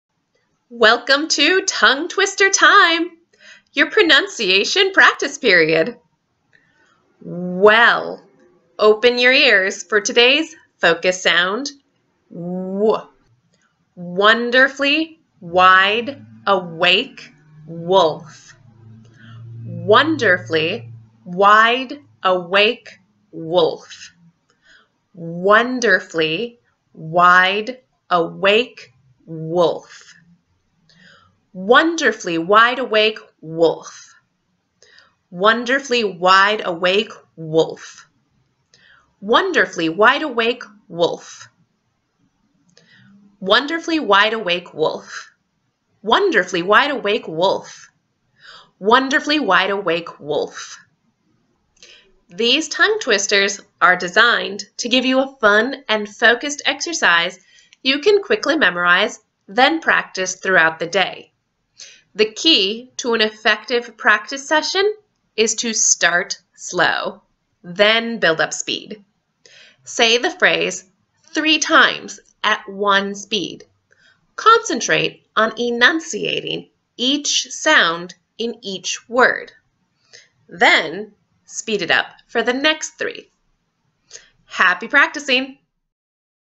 Tongue Twister Time Ep 29: Wonderfully Wide Awake Wolf
Learning individual sounds is the first step to speaking clearly, but language involves combining sounds, which means needing to train our tongues to move from one sound to another very quickly. These tongue twisters are designed to give you a fun and focused exercise that you can quickly memorize and practice throughout the day. The key to working with these is to start slow and build up speed.

For a personalized pronunciation plan, come on over to craftingsoundmeaning.com